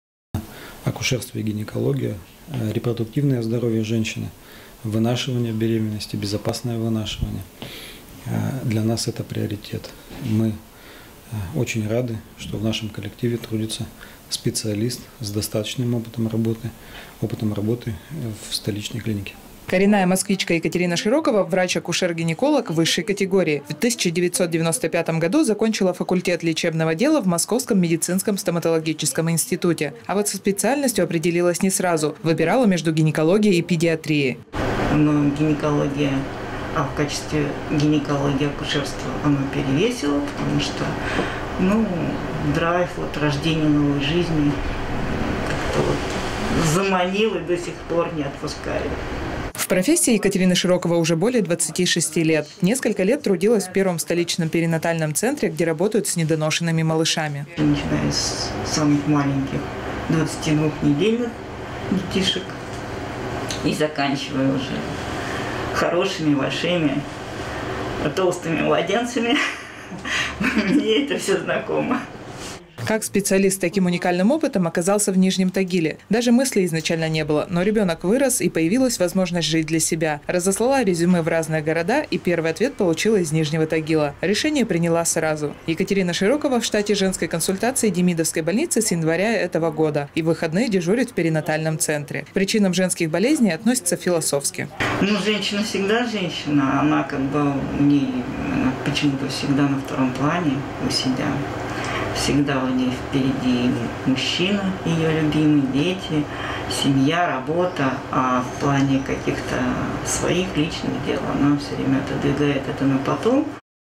Московский опыт
Этот праздник официально утвержден в 1980 году. Указ отмечать день медицинского работника каждое третье воскресенье июня принял Президиум Верховного Совета СССР. В этом году праздник выпал на 20 число. К этой дате на "Тагил-ТВ" мы готовим серию репортажей о врачах. Уникальная история из женской консультации Демидовской городской больницы. Здесь работает московский акушер-гинеколог со стажем.